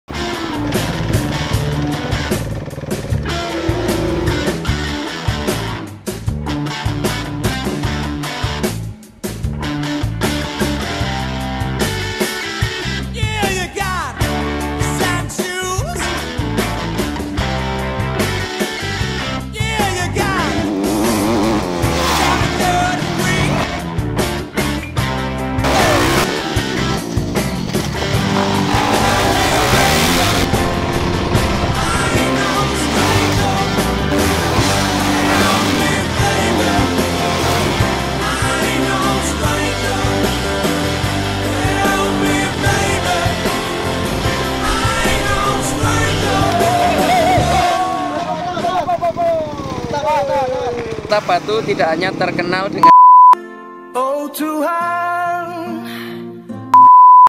Wisata MOTOCROSS Kota BATU
Sirkuit Motocross di Kota Batu
tepatnya di Daerah Jalibar, dari museum angkut ambil arah ke panderman hill menuju arah Coban Rais ,, di perjalanan anda akan menemukan sirkuit ini di kiri jalan.
akses menuju kesini cukup mudah , hanya membutuhkan 10 menit dari jantung kota Batu.